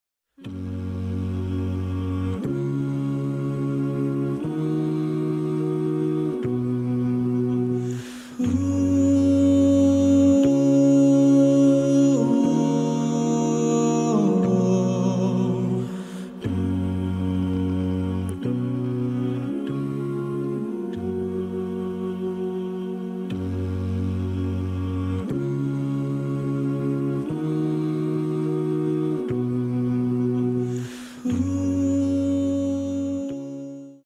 Lapsi nuk shkruan për tre persona
“Lapsi është ngritur për tre persona (nuk ka detyrim për tre grupe personash): personi që fle derisa të zgjohet; fëmija derisa të arrijë moshën e pubertetit dhe personi që nuk është i shëndoshë mendërisht, derisa ai të shërohet”